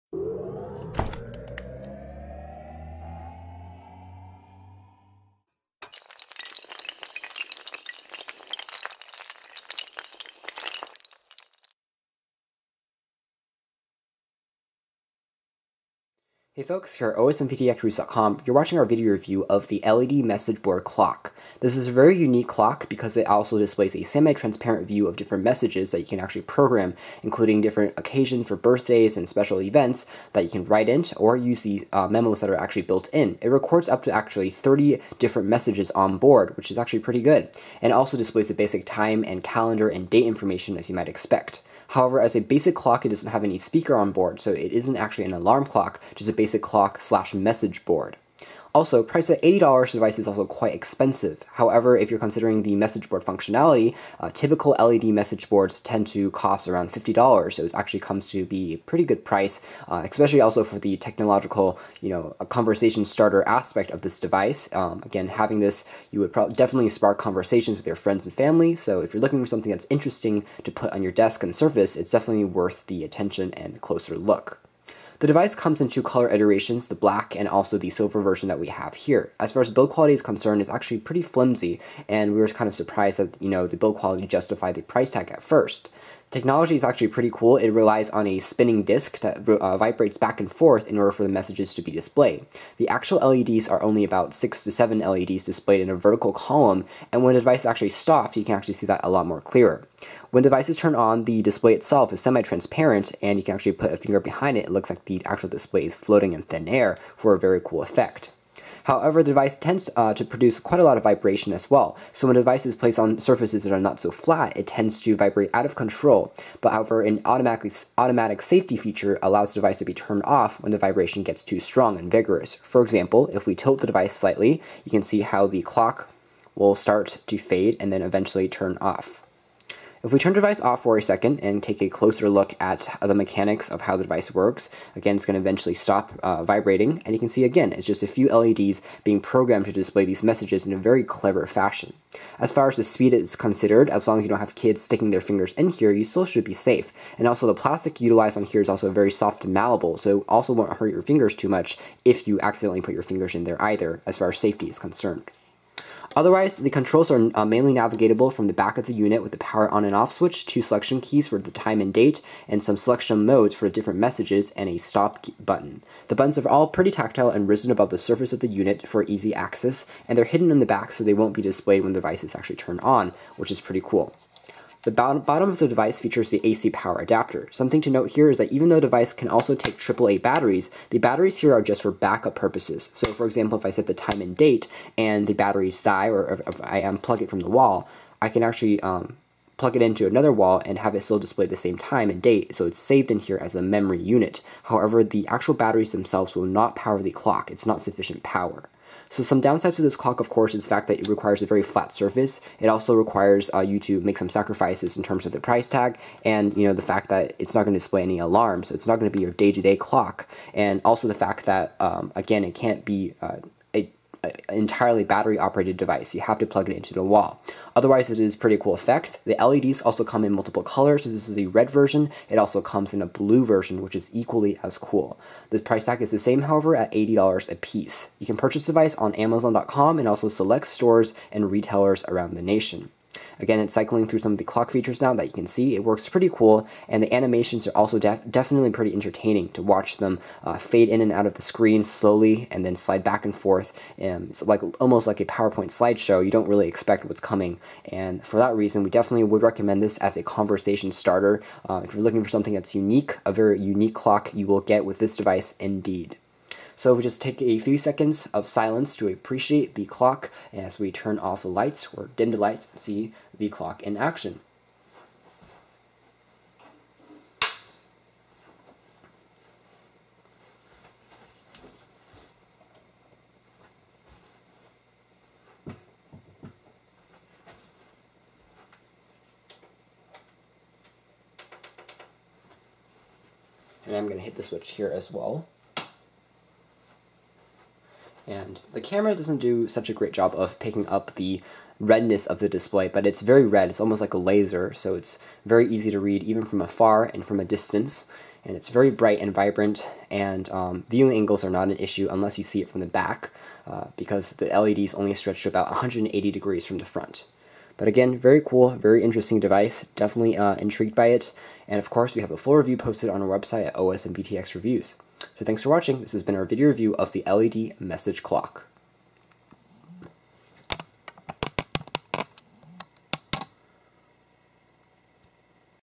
Floating LED Message Clock Review:
A programmable LED clock that acts as a digital bulletin board, while displaying time and date information. We take a closer look at the futuristic LED message clock.

OSReviews
OSTechNews